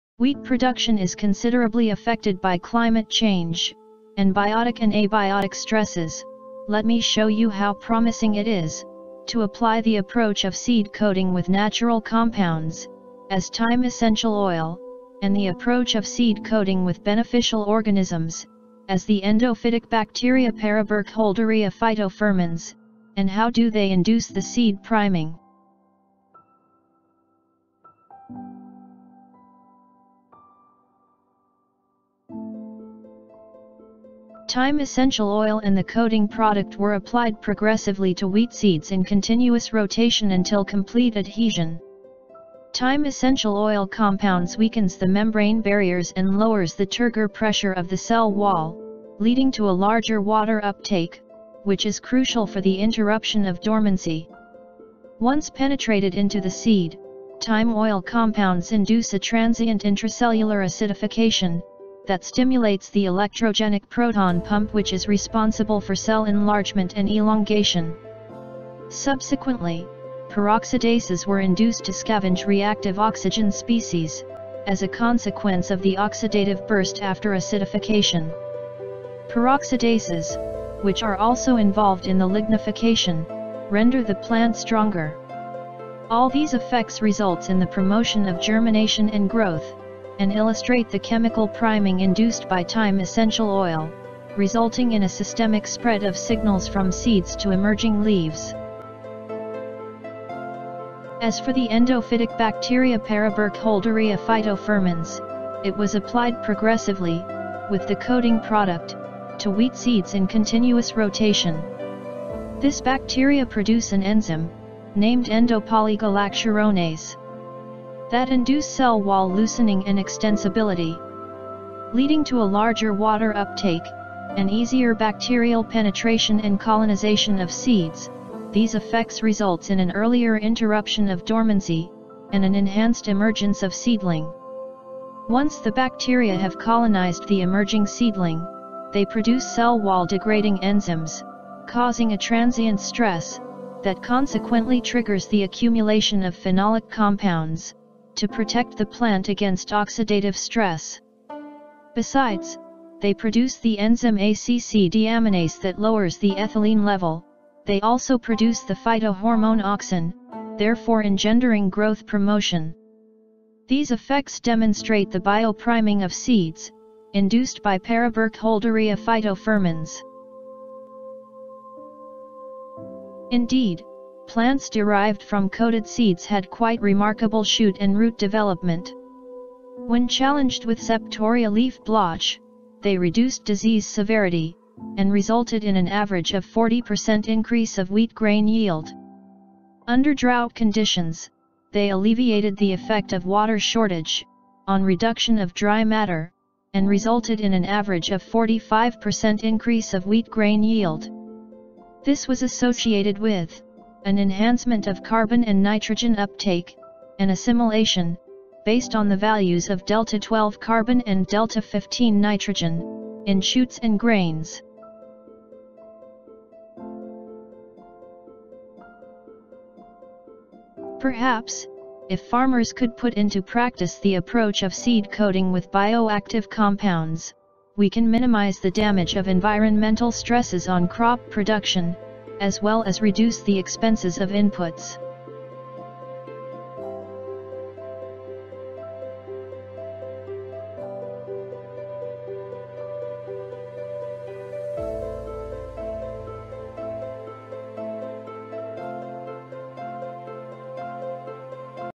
seed biopriming and chemical priming
Seed coating with Paraburkholderia phytofirmans and thyme essential oil for improving resistance to Septoria leaf blotch and drought stress in durum wheat